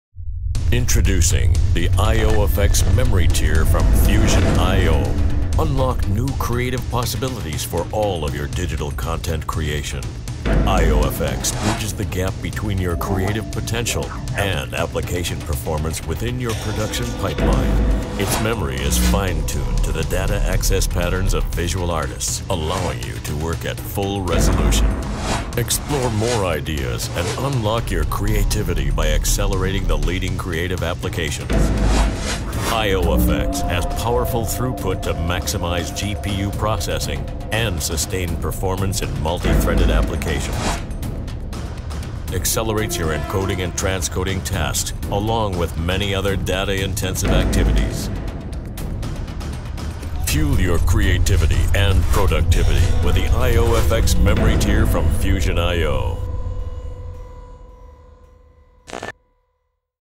Mine and Doug Urquhart's entry in the ioFX contest.
This is mine and Doug's entry in the ioFX's contest. We placed in the top 10, but didnt make it to the top 3.

All done in Cinema 4D. Post work in AE.